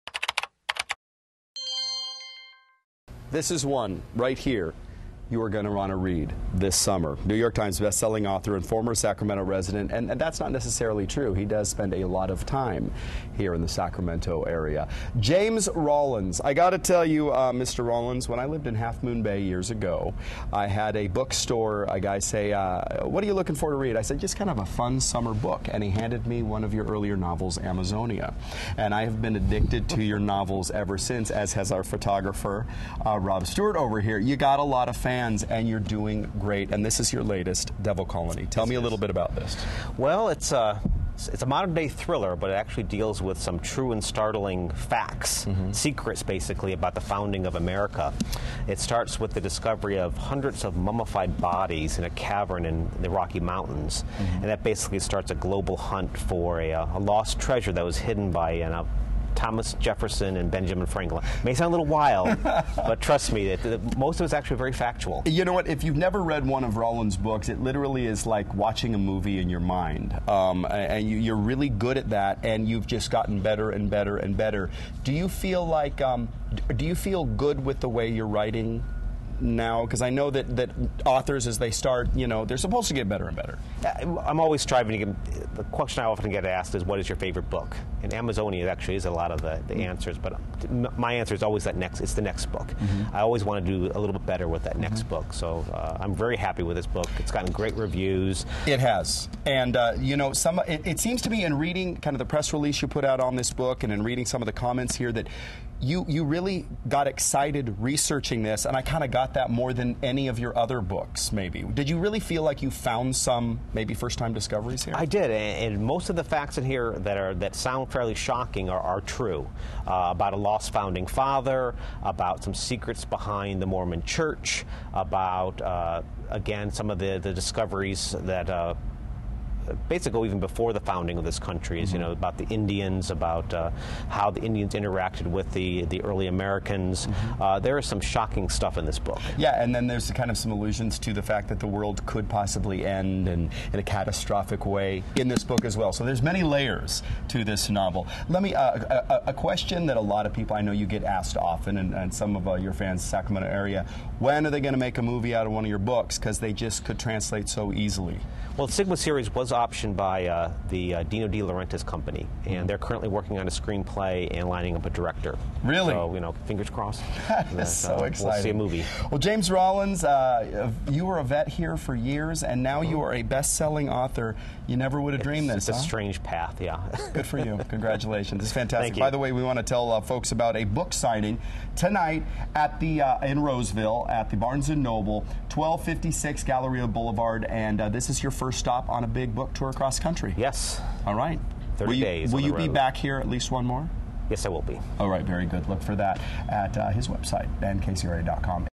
James Rollins Discusses 'The Devil Colony'
KCRA 3's Teo Torres chats with author James Rollins about his new book called "The Devil Colony."